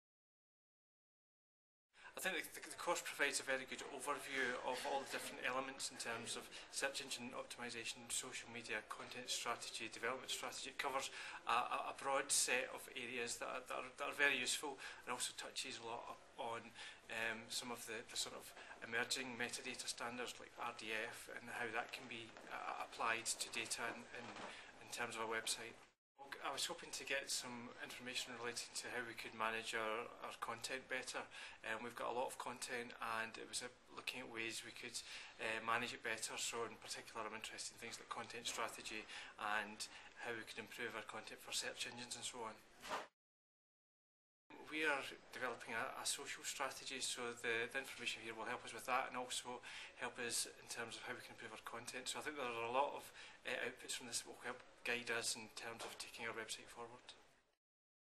Interview with Stuart McLaren, SQA
Stuart McLaren, Webmaster at the Scottish Qualifications Authority, interviewed at the 'Maximising Online Resource Effectiveness' workshop in Glasgow.